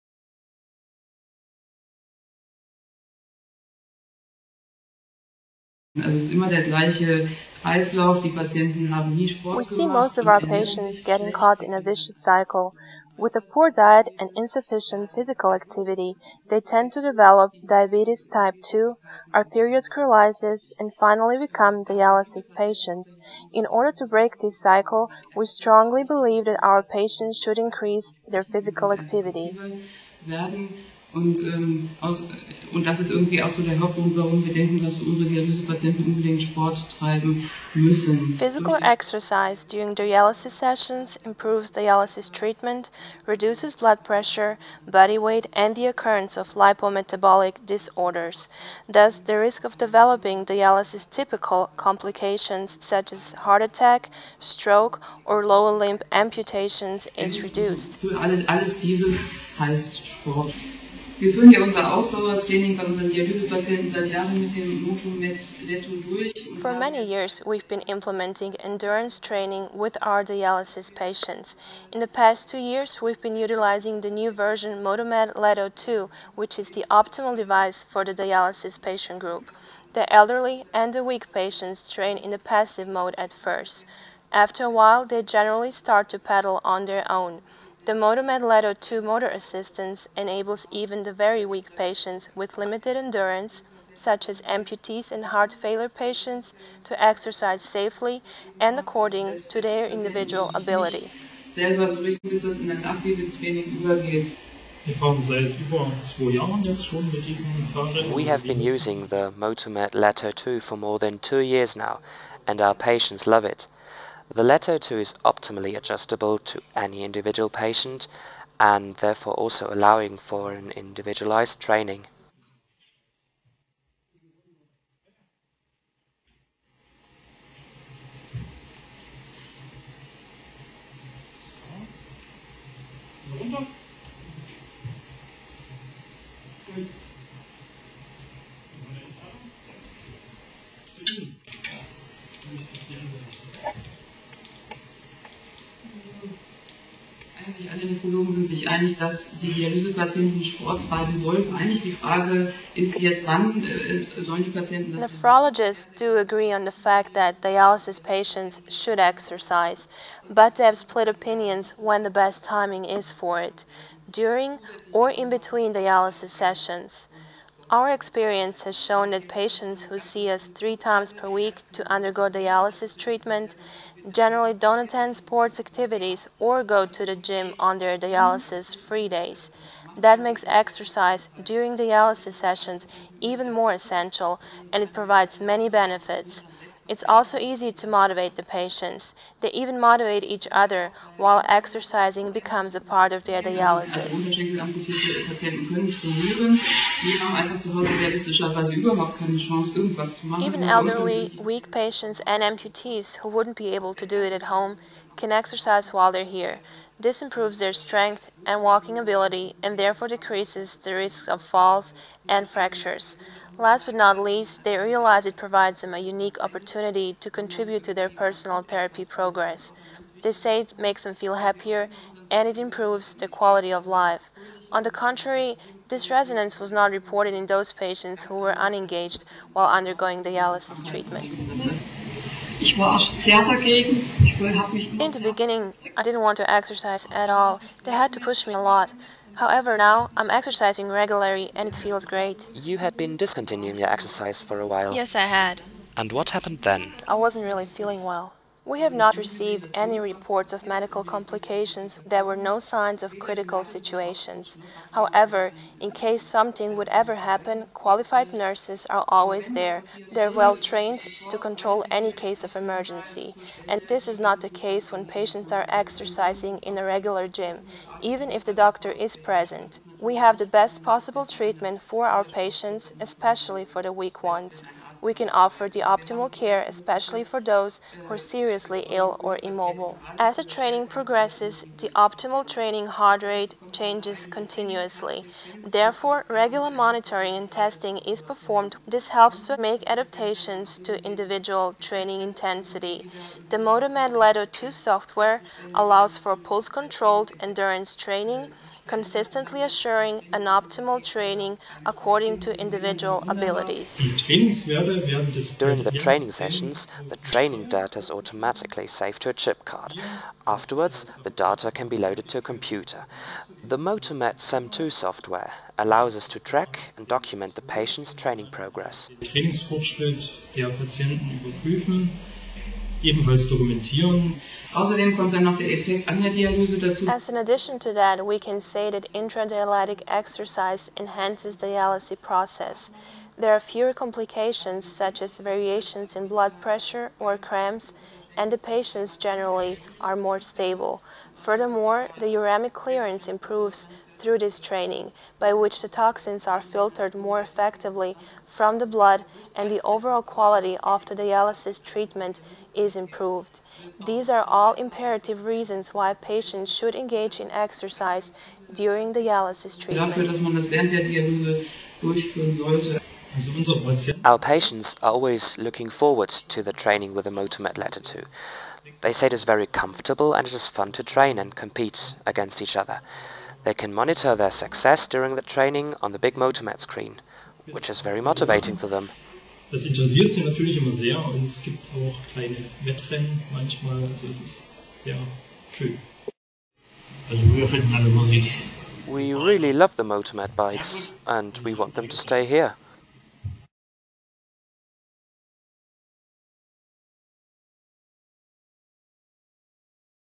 MOTOmed letto2 -- Movement Therapy During Dialysis
Dr.med Kirsten Anding-Rost, leading doctor at the KfH Dialysis Center Bischofswerda, Germany, is talking about the impact of MOTOmed letto2 movement therapy during dialysis treatment.